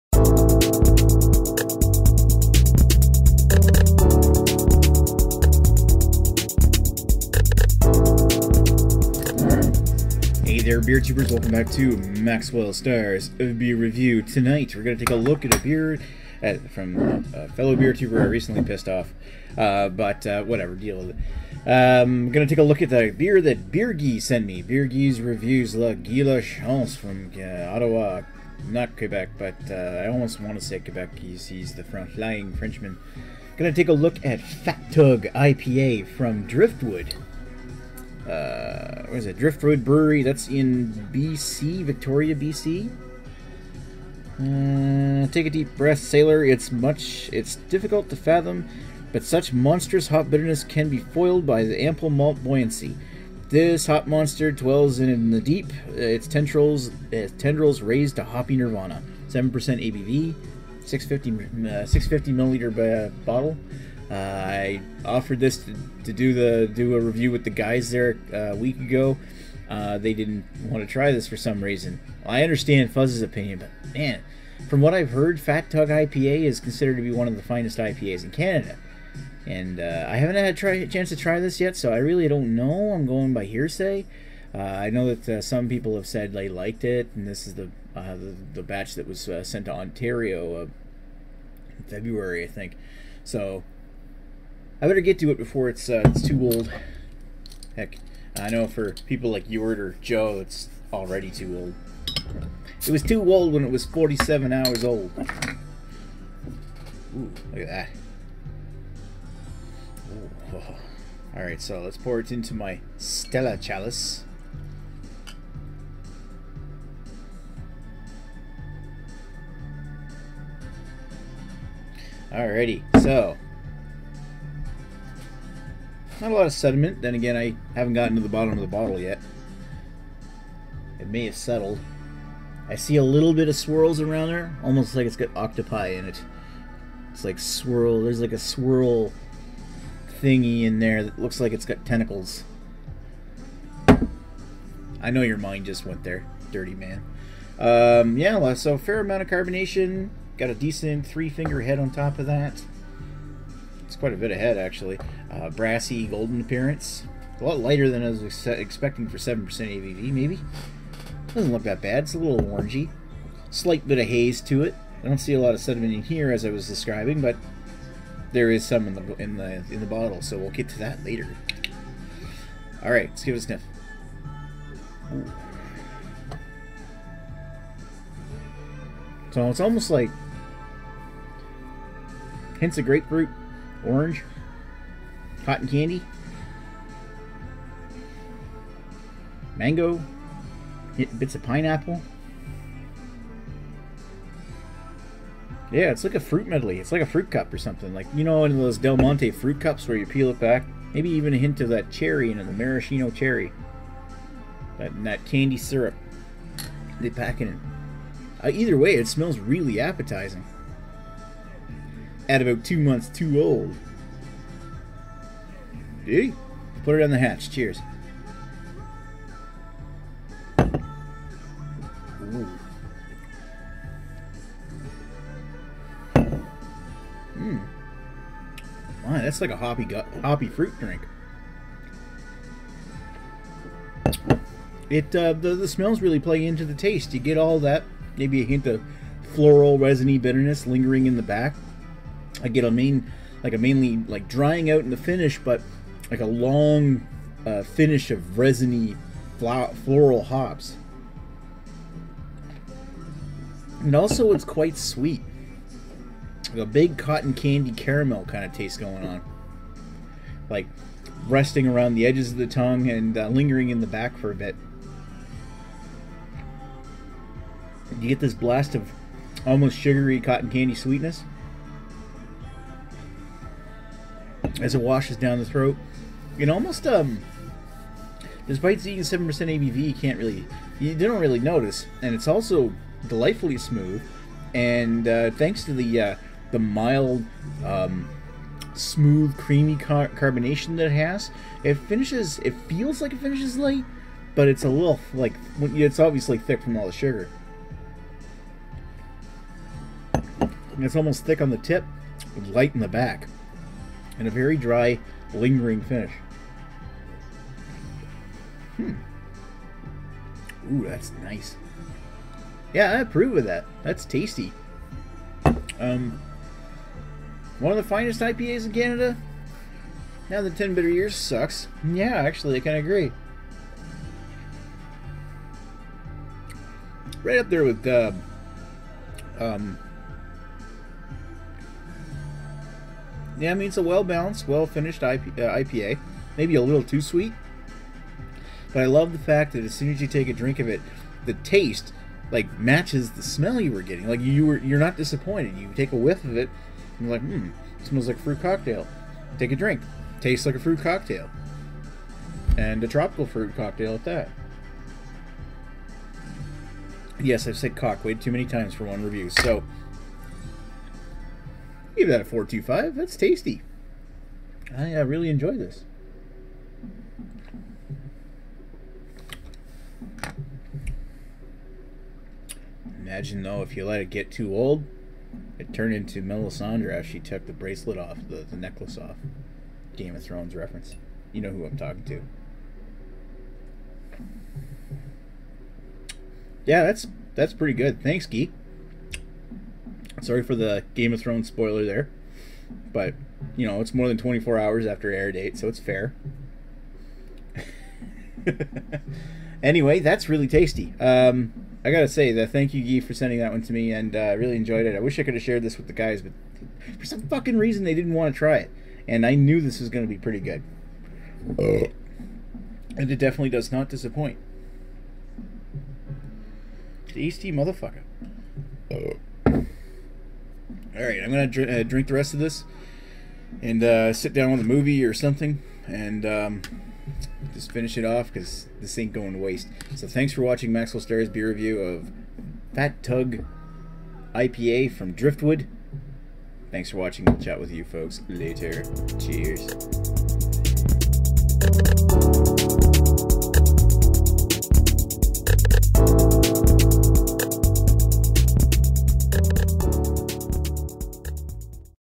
Driftwood Fat Tug IPA - #852 - Maxwell Starr's Beer Review
Going solo to check out an IPA from BC, sent to me by Beer Guy Reviews. Cheers!